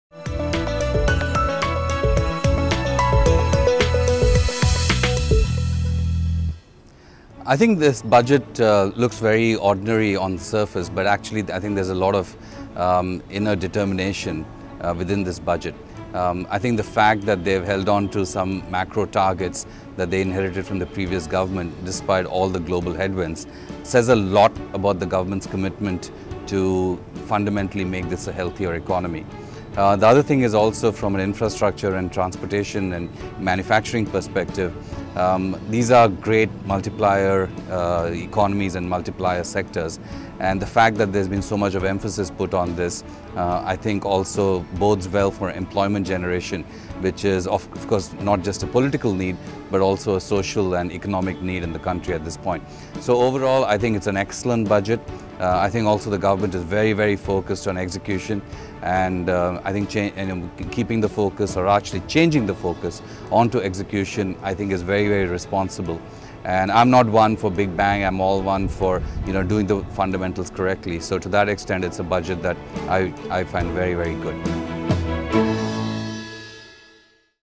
​Skoda CEO: Did the Budget deliver? | Mint CEO30
Mint hosted an exclusive breakfast for CEOs across industries to discuss whether the budget delivered or not. Sudhir Rao, CEO of Skoda, gives his perspective on the announcement of the 2014 budget.